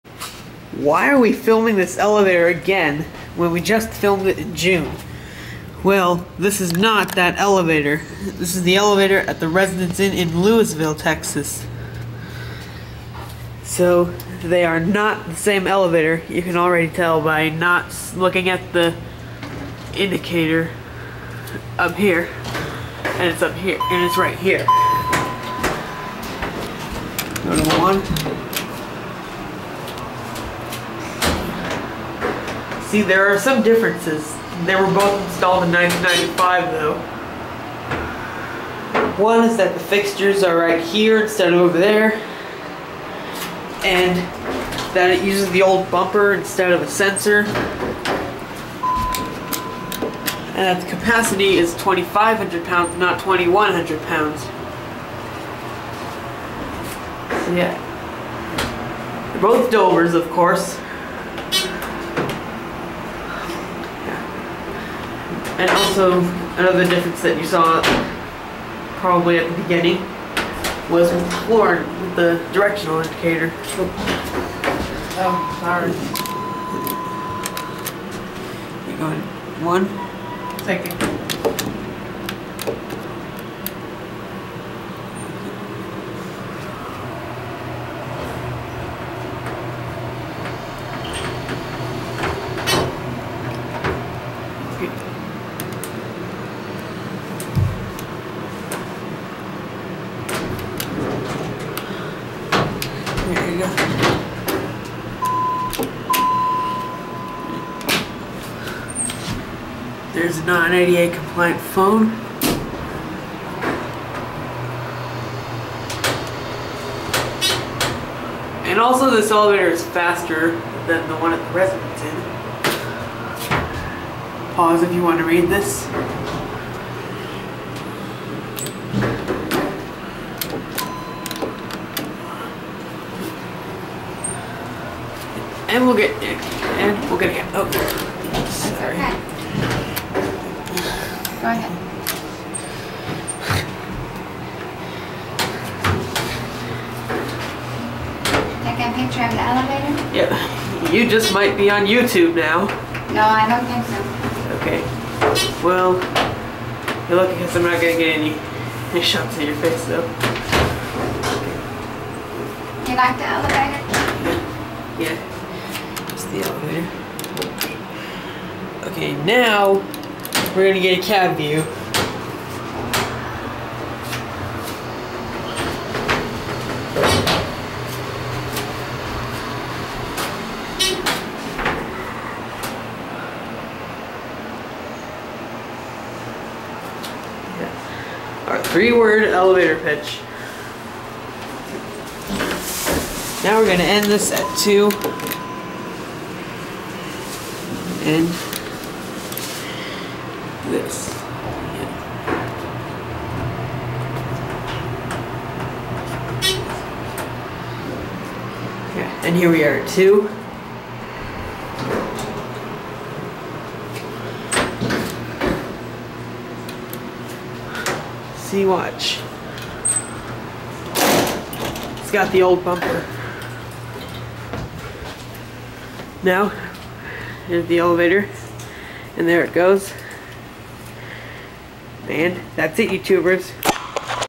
Dover Hydraulic Elevator at Residence Inn Lewisville TX
This is the hotel I'm currently staying in. It has a MUCH better wireless network, unlike the Courtyard in downtown Amarillo... Anyways, this elevator is similar to the Amarillo one, but there are some differences. Luckily I didn't get any shots of that cleaner lol :3